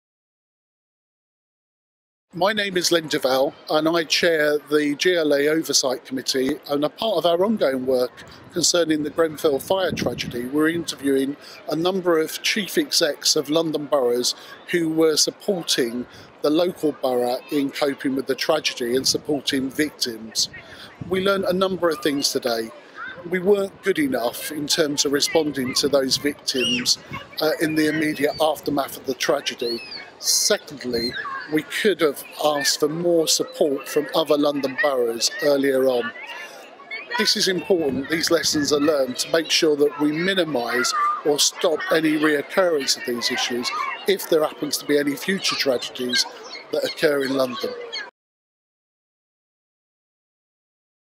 Len Duvall AM, Chair of the Oversight Committee, on lessons learnt from the Grenfell Tower fire
The GLA Oversight Committee questioned local government representatives and the Mayor’s Office about their views of the response from the London resilience forum to the Grenfell Tower fire.

The guests are:

• Barry Quirk, Chief Executive, Kensington and Chelsea
• Doug Patterson, Chief Executive, Bromley
• Eleanor Kelly, Chief Executive, Southwark
• David Bellamy, Chief of Staff, Mayor’s Office